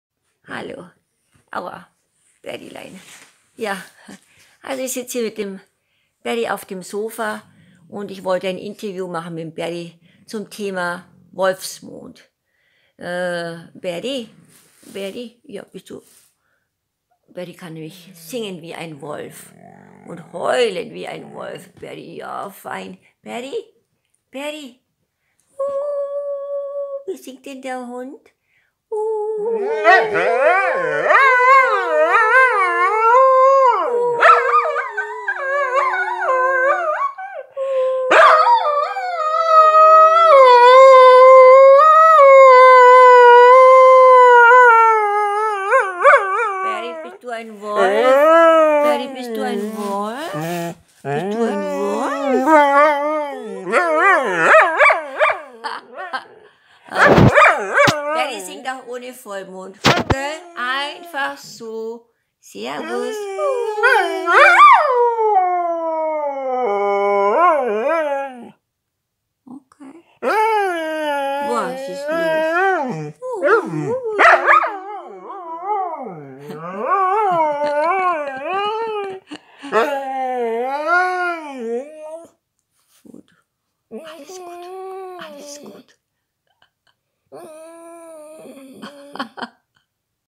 Berry singt den Wolfsmond
Hund Berry braucht keinen Vollmond, oder wie er im 1.Monat des Jahres heisst,  "Wolfsmond" (18.1.22) um loszuheulen, wie ein Wolf. Es klappt auch auf dem Sofa, am Tag danach.